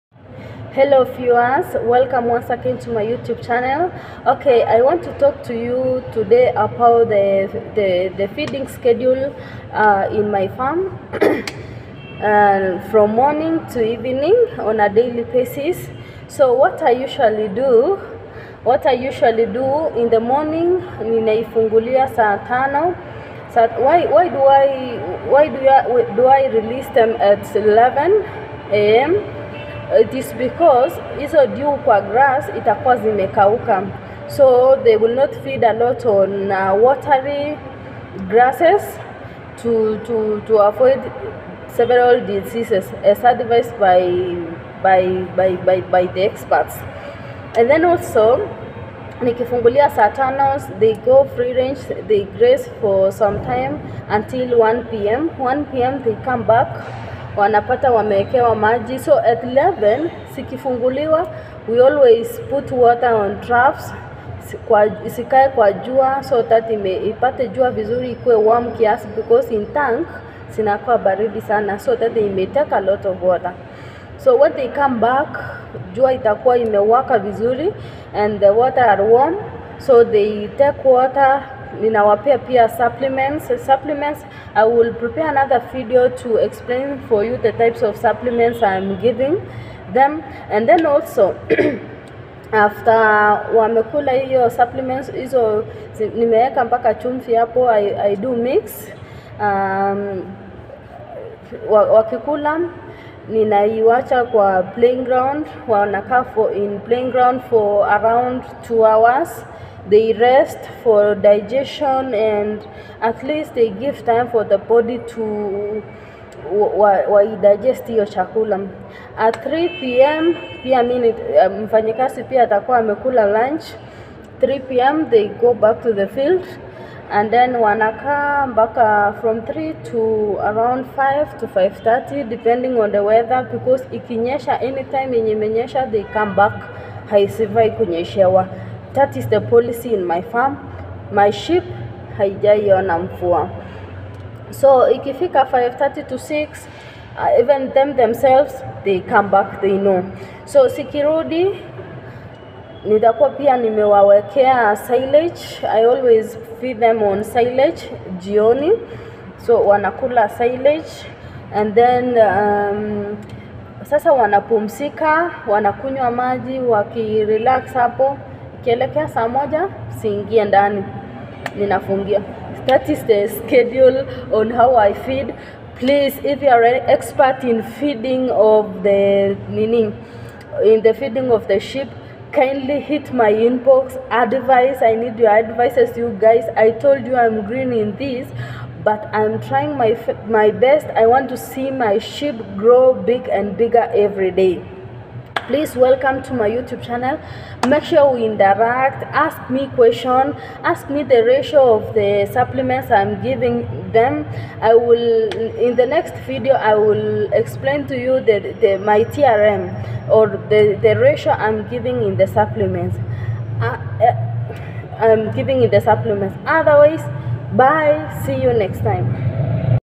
what is daily feeding schedule dorper sheep
feeding :
Free range
silage
supliments
mineral salt